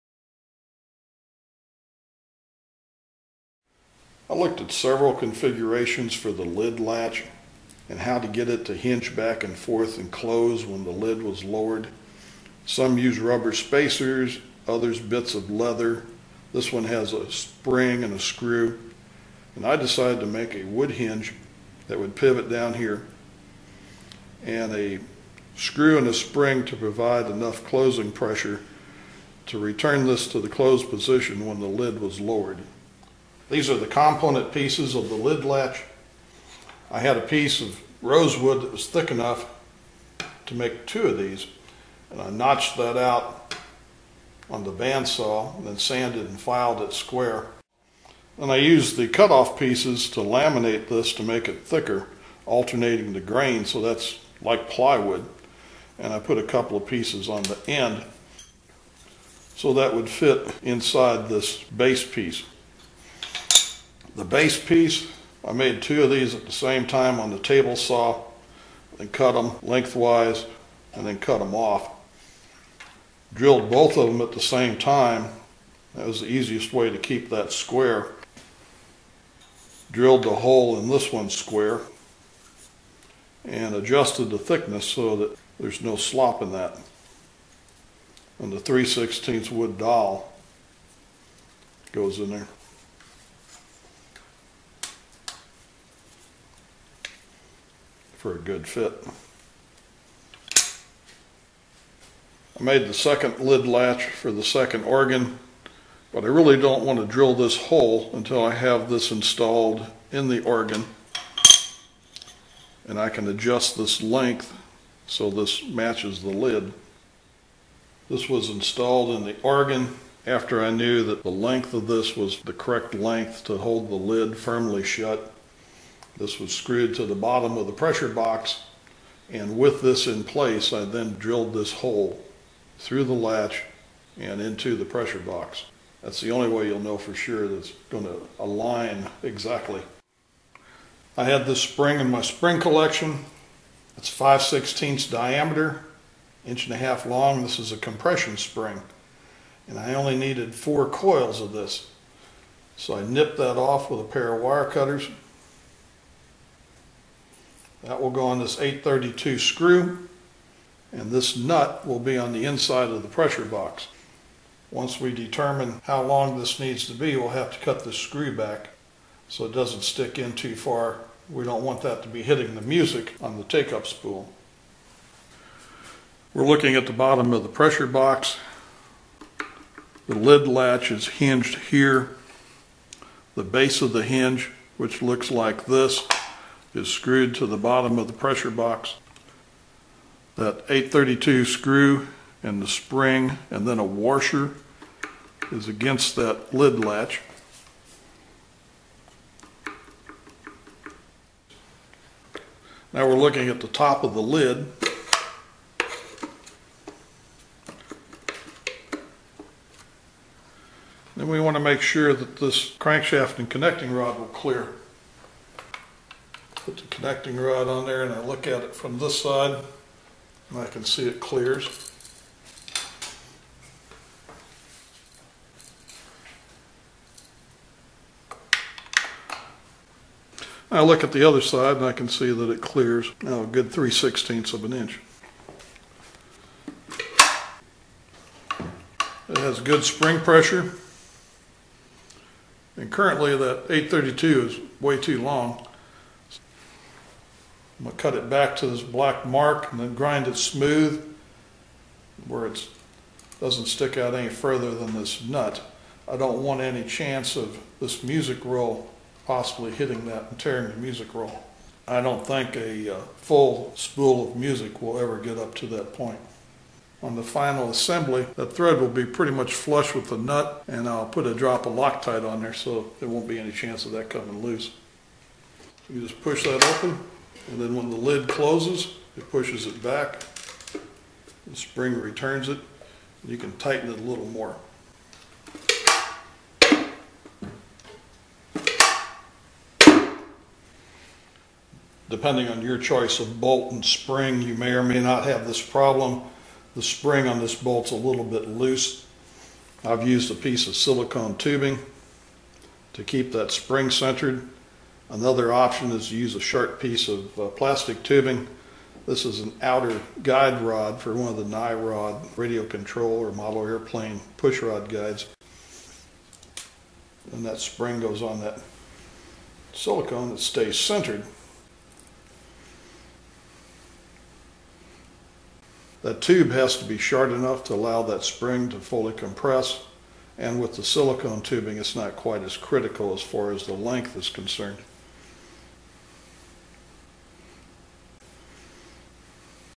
Senior 20 Organ (18) Pressure Box Lid Latch
Seemingly simple, this is in a crowded area with the crankshaft, connecting rod and the clutch idler disc support rod.